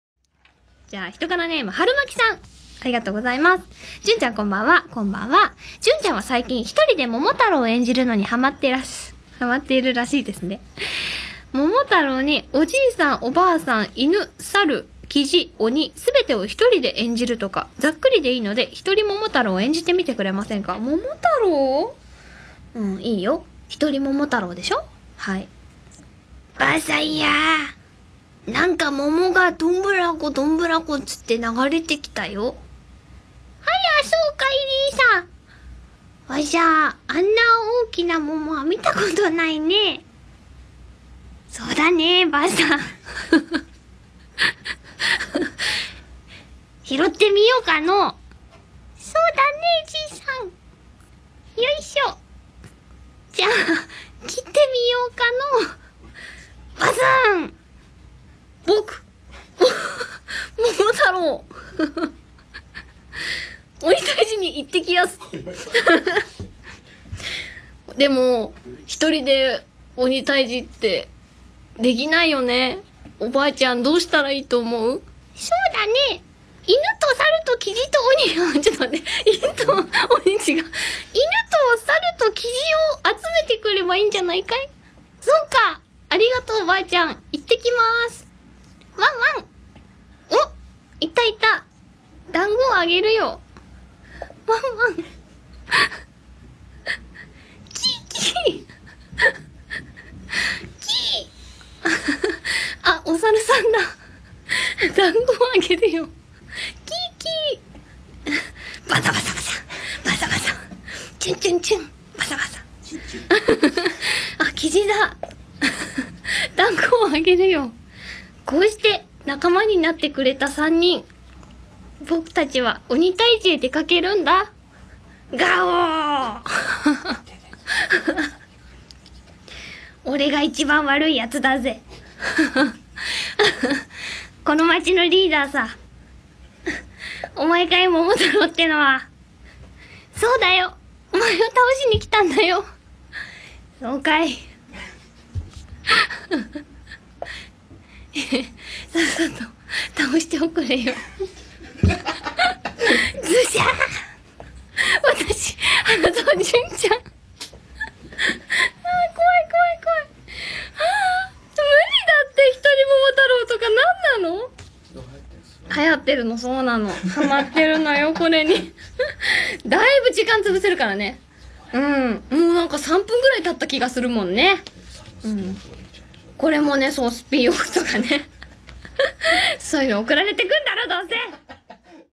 花澤香菜 「コワイコワイ、無理だって」 壮絶！ ザーサンの一人桃太郎
ざーさん・・・いったい何と戦ってるんだ・・・(´・ω・｀)